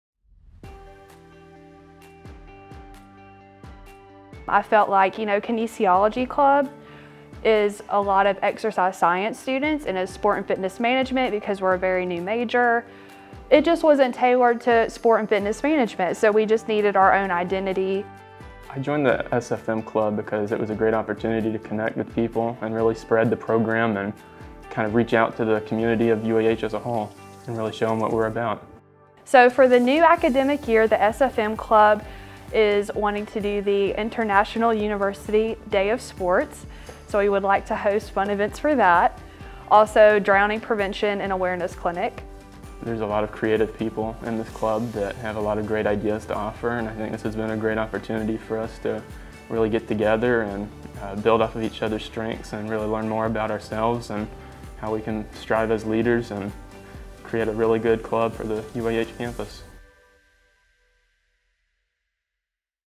Sports and Fitness Management Club
Welcome! The Sports and Fitness Management (SFM) program is designed for those interested in the administrative aspect of sport and fitness. The purpose of the Sport and Fitness Management degree is to prepare students for both graduate studies and entry into management, marketing, and administrative positions in educational and sport business settings.